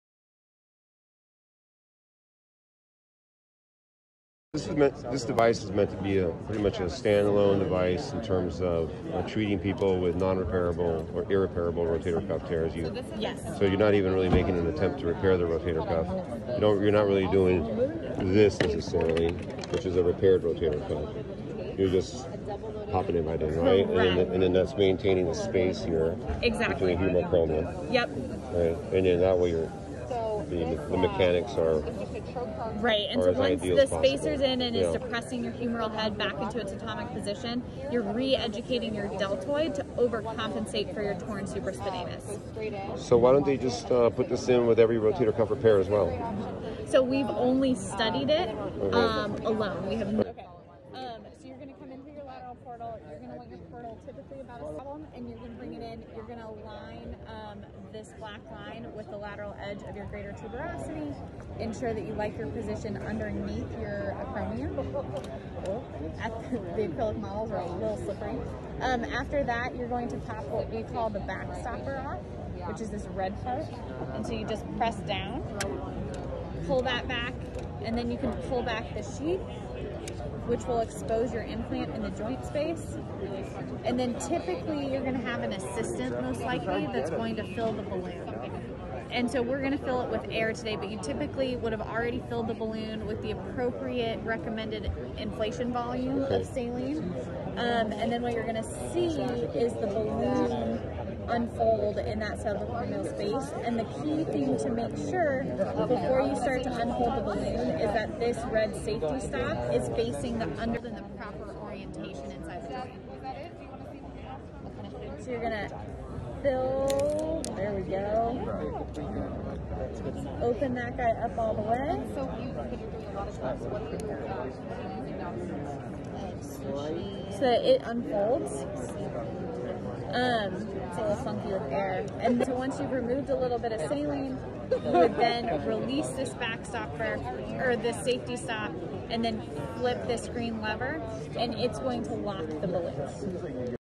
A New Device for IRREPARABLE Rotator Cuff Tears- The Stryker InSpace
This balloon-like device is surgically placed in between the humeral head and the acromion bone of the shoulder subacromial space. It acts as a spacer or in other words it pushes the humeral head down. By pushing the humeral head down the length of the deltoid muscle of the outer shoulder is reestablished allowing it to function again.  In essence this balloon will change the mechanics of the damaged shoulder so that movement of the arm does not rely on the damaged rotator cuff tendon but rather on the better positioned deltoid muscle￼.

After insertion and recovery patients have a better chance of being able to raise their arm again by using the deltoid rather than the absent rotator cuff tendon.￼  Patient will still have to go through physical therapy program to retrain the deltoid￼ muscle.

Alternative options for irreparable rotator cuff tears are much more invasive￼ and include superior capsular reconstruction as well as reverse shoulder replacement.

The Striker InSpace balloon is relatively easy to insert and the potential complications are relatively low.￼￼